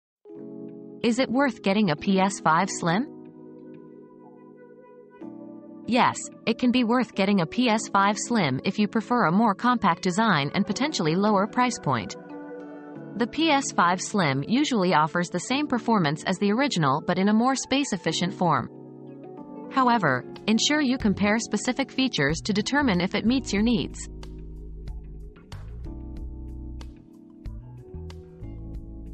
Is it worth getting a PS5 Slim?
Is the PS5 Slim Worth It? Pros and Cons Revealed! • PS5 Slim Review • Discover if the PS5 Slim is worth the investment with this informative review. Learn about the potential benefits of a more compact design and lower price point, but make sure to compare features to ensure it meets your gaming needs.